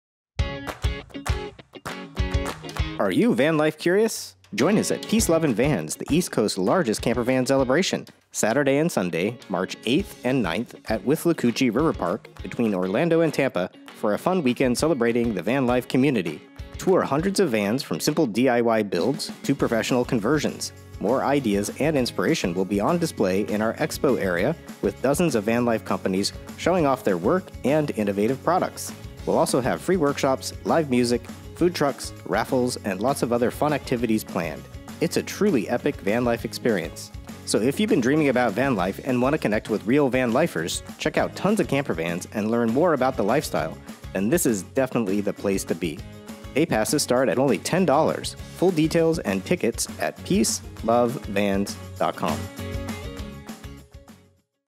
Peace Love & Vans - Florida Camper Van Gathering - March 2025
Whether you’re a vanlifer or just vanlife curious, come and join us at the most epic vanlife meetup and industry expo in sunny Florida and one of the largest in the Southeast USA!

Together, we’ll form a 1960’s inspired utopian van village for the weekend featuring hundreds of vans ranging from DIY builds to professionally converted vans and everything in between.

The jam-packed schedule is filled with van tours, expert-led workshops, food trucks, nearly $15,000 in raffle prizes, live music, and an awesome community of like-minded, adventurous people!

There will also be dozens of vendors and exhibitors showing off vans, products for vans, creative crafts, and more! Also, our sponsor, Victron Energy will be bringing their training trailer to showcase their state-of-the-art mobile power systems and their experts will be on-site to answer any questions you might have.

The event is hosted at the amazing Withlacoochee River Park on a wooded site that features ancient oak trees and a grassy field. And, if you want to take a break from vans, you can take advantage of awesome outdoor activities like hikes, biking, and enjoying the river. The park is located between Tampa & Orlando (about 40 minutes from each city).